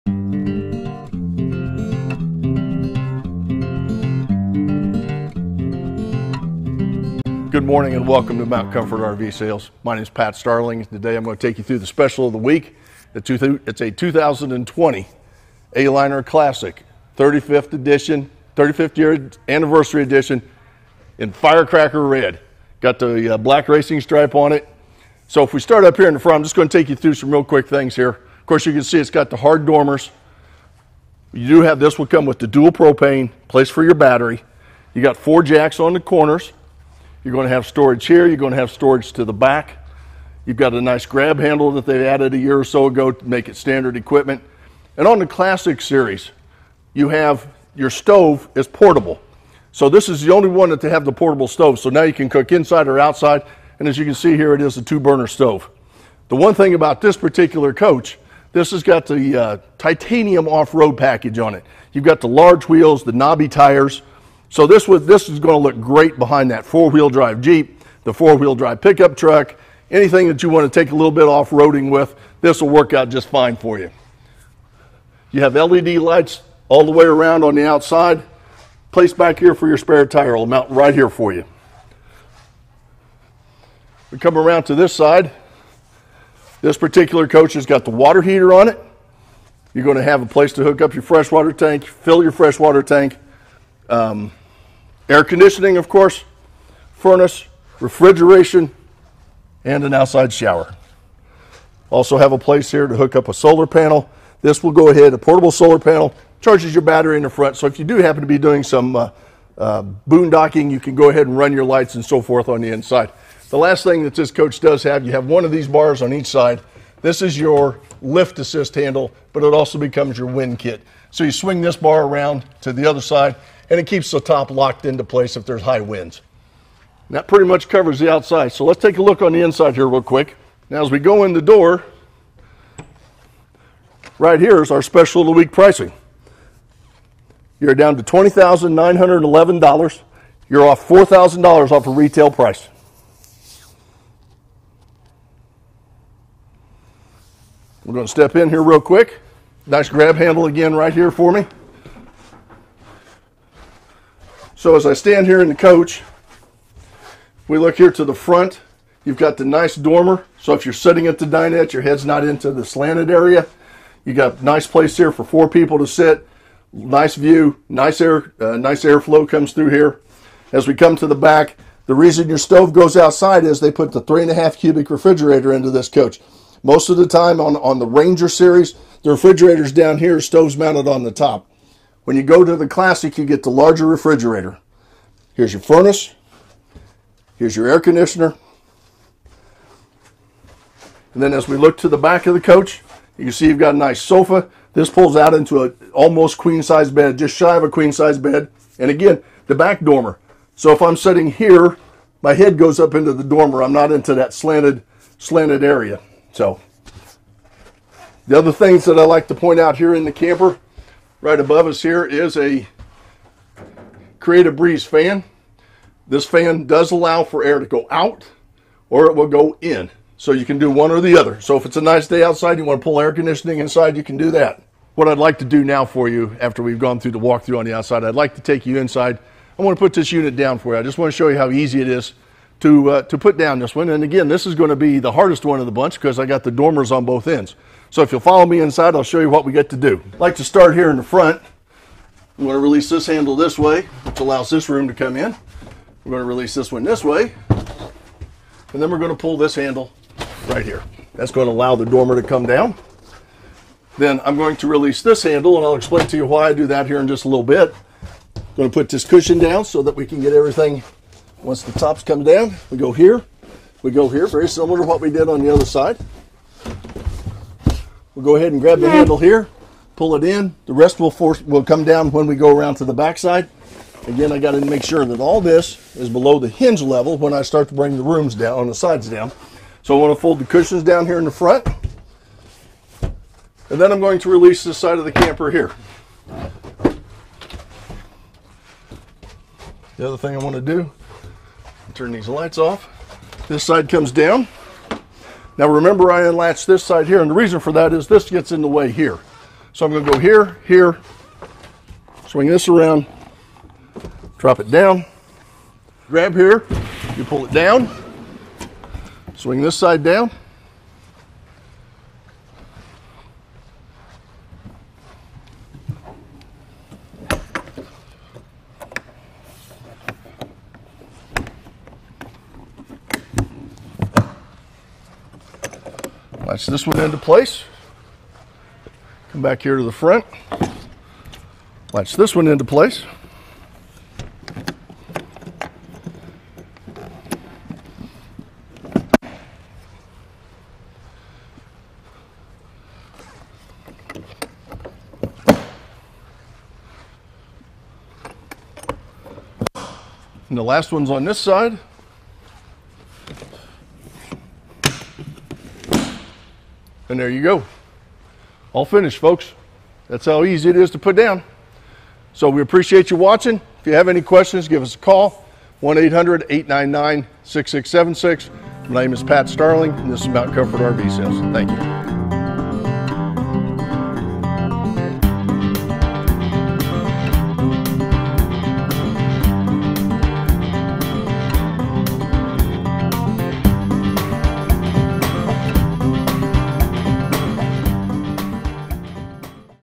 HOT DEAL 2020 Aliner Classic Red | Price Good Till 3-12
This week's HOT DEAL OF THE WEEK is a candy apple red 2020 Aliner Classic 35th Anniversary pop-up camper. Take a video tour with Pat Starling at our Indiana RV Dealership. Hurry, this pricing only lasts 7 days!